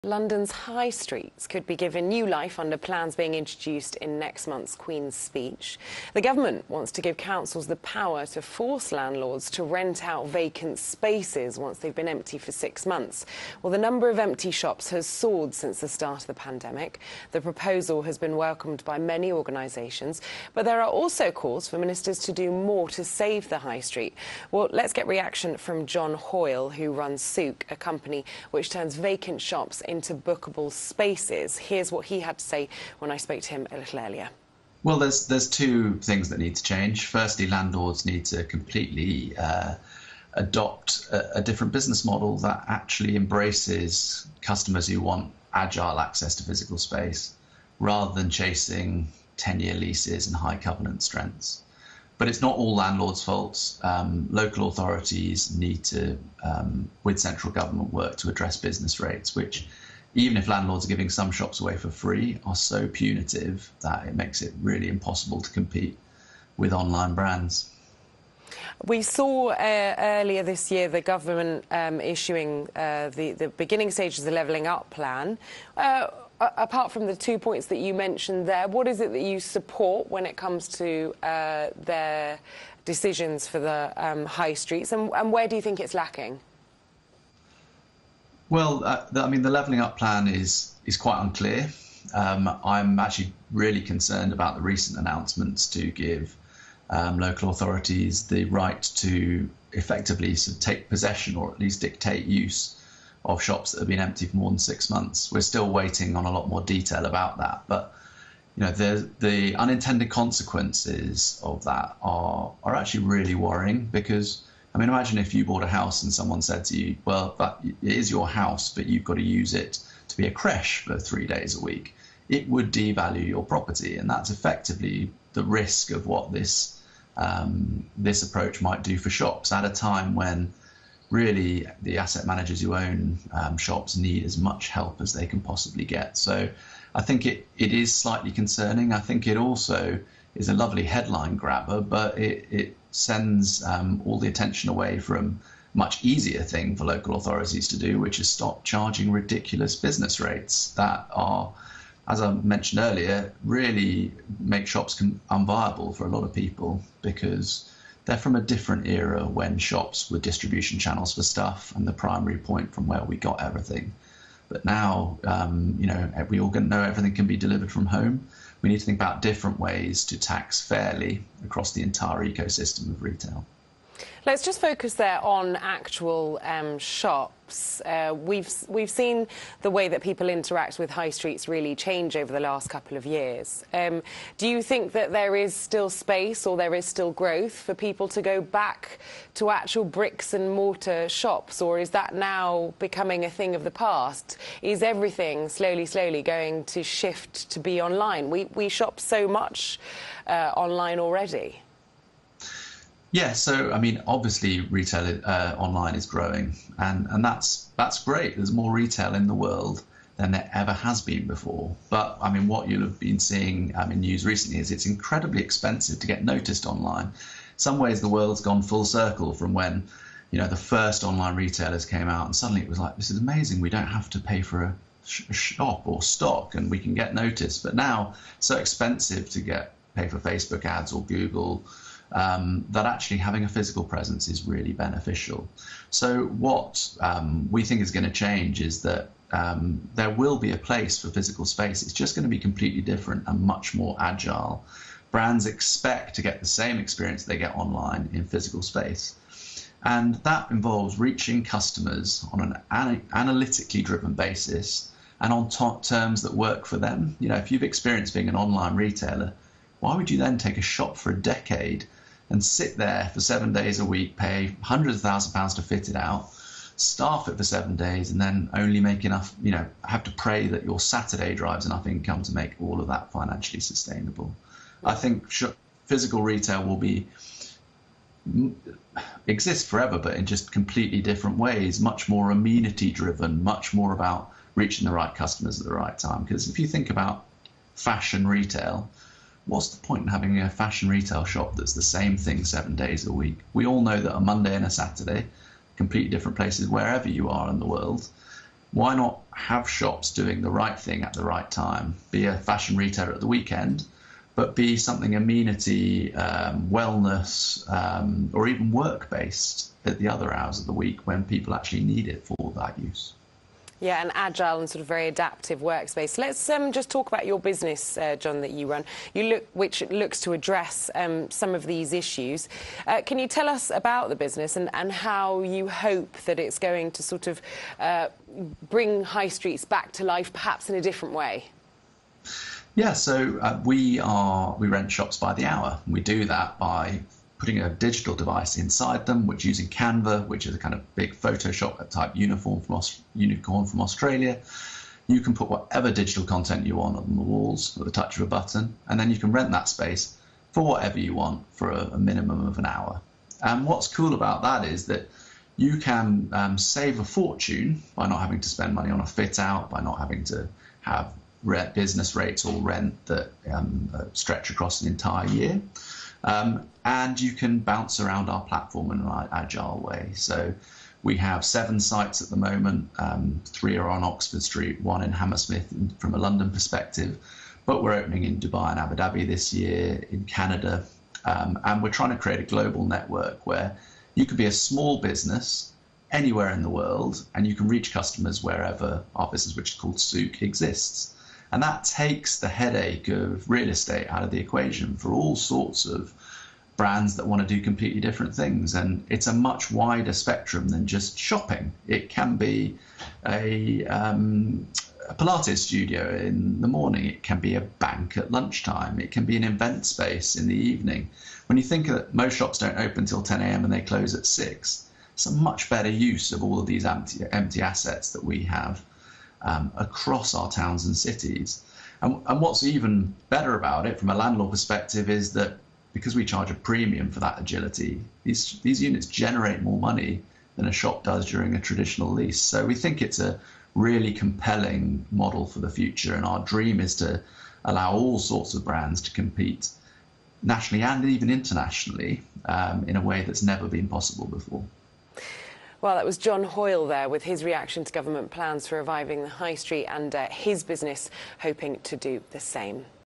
Levelling Up: Sook CEO John Hoyle on London Live
John Hoyle speaks to London Live on the potential impact of the government's Levelling Up announcement to 'force' landlords to let out vacant shops; the future of online shopping; and what all of this means for bricks and mortar retail and physical space.